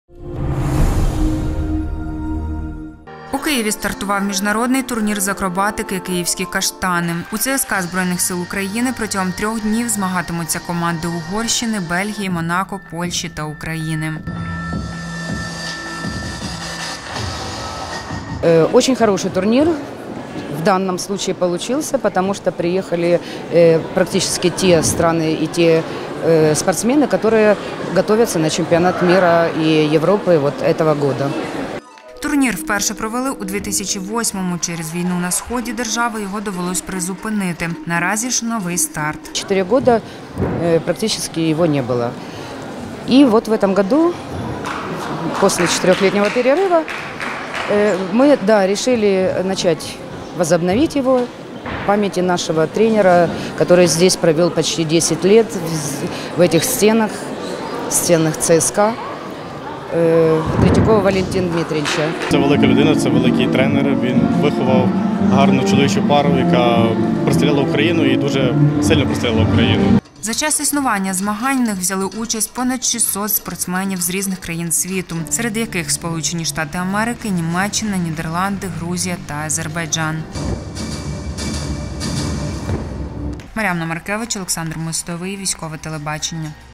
У Києві стартував міжнародний турнір з акробатики
У Києві стартував міжнародний турнір з акробатики «Київські каштани». У ЦСК Збройних сил України протягом трьох днів змагатимуться команди Угорщини, Бельгії, Монако, Польщі та України.